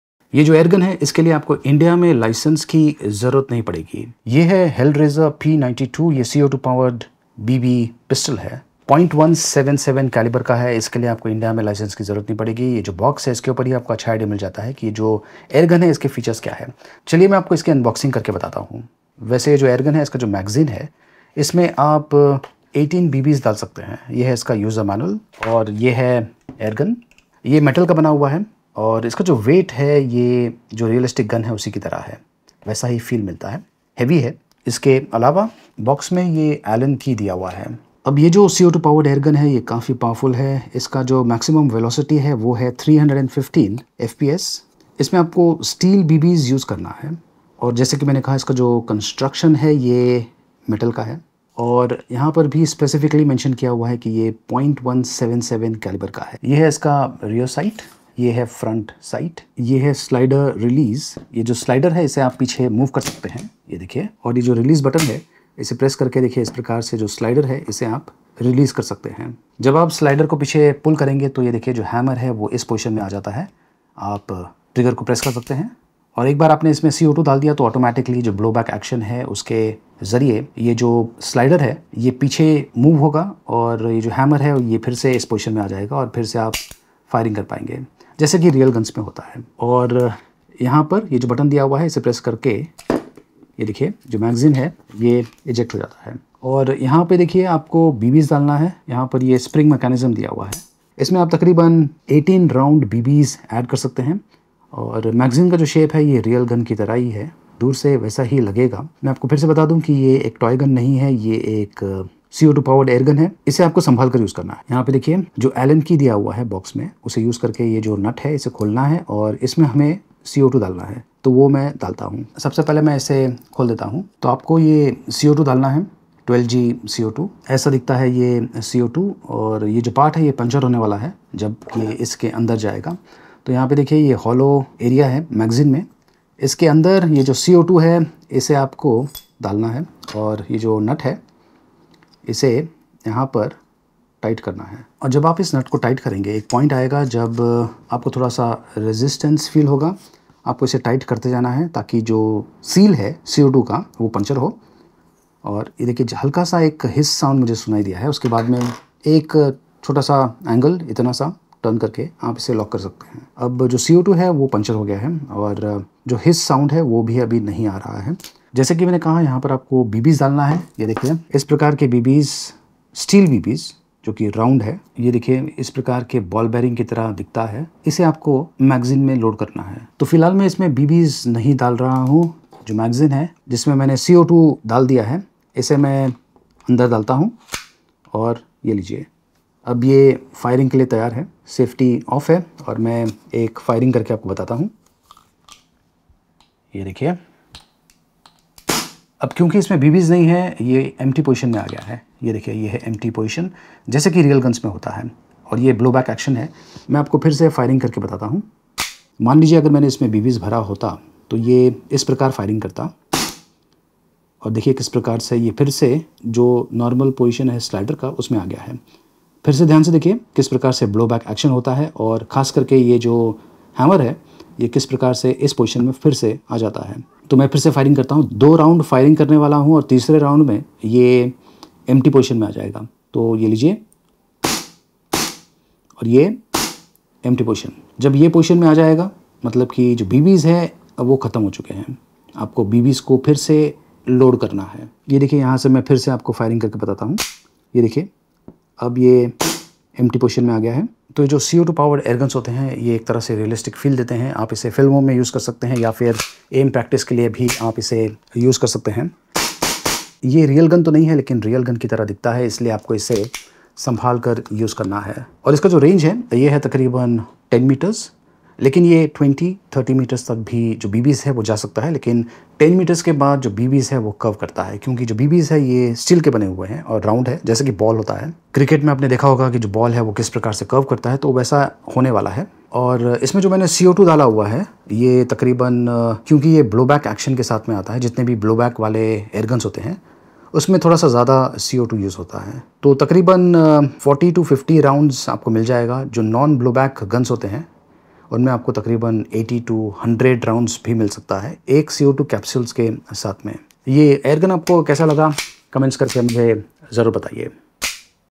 Blowback co2 powered air gun the Hellraiser P92 (.177 cal) No License required
This is a blowback Hellraiser P92 air gun, this is .177 cal and no license required. Here is the P92 unboxing and features. This is not a real gun, this is not a toy, so handle it with care.